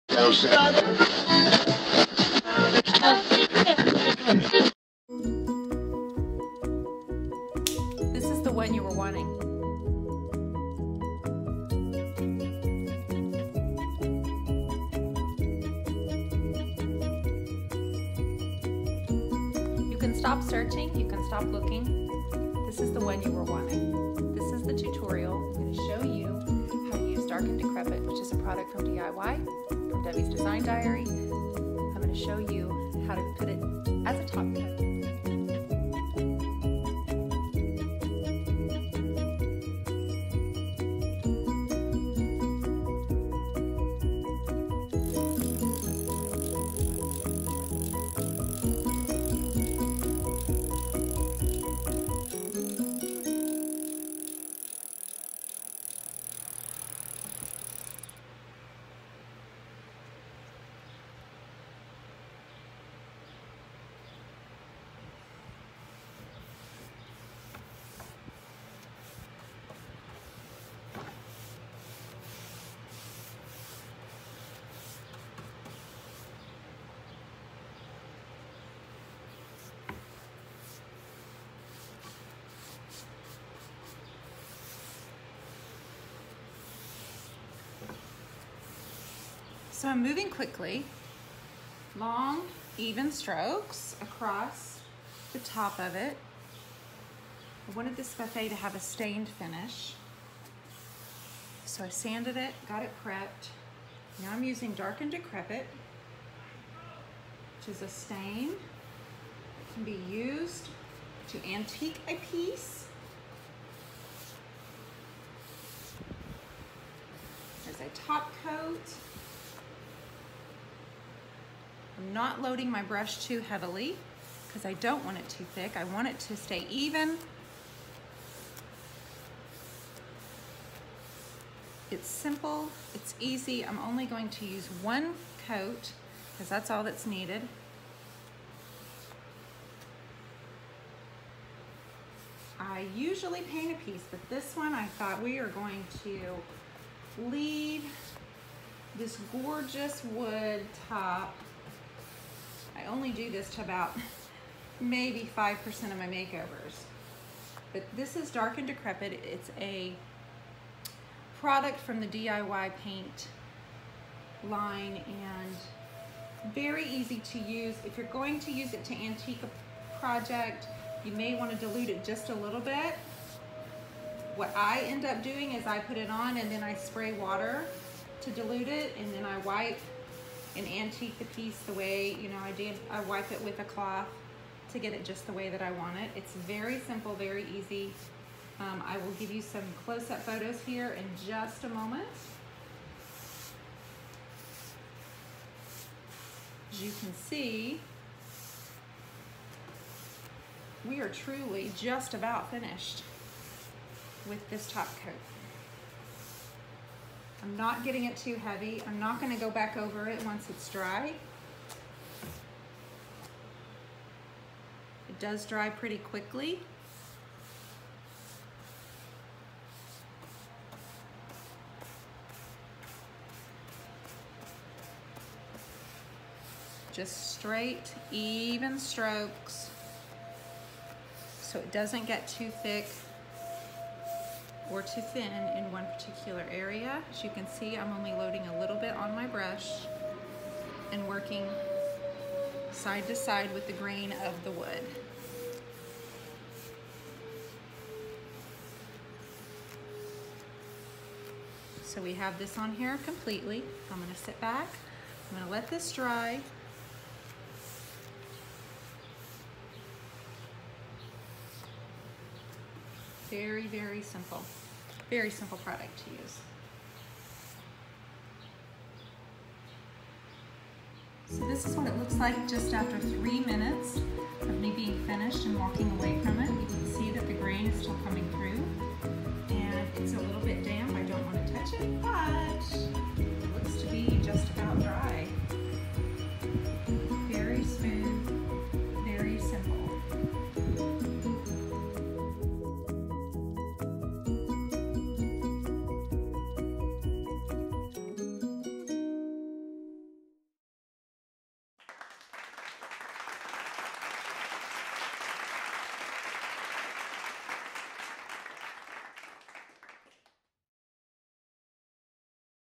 Furniture Makeover How To Refinish the Top Surface of a Buffet - Easy as 1,2,3
Using a product called Dark & Decrepit from DIY Paint, I show you how easy it is to create a smooth, seamless finish.